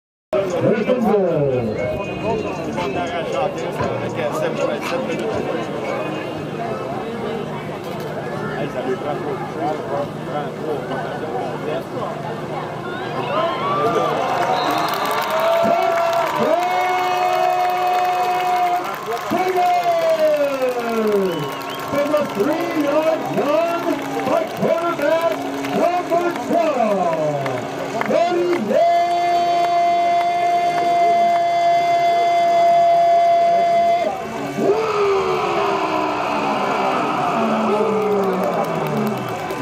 Concordia Stingers QB Olivier Roy runs for a 3-yard TD vs. McGill Redbirds 9/17/22
This touchdown increased the Stingers' lead to 35-16 midway through the 4th quarter.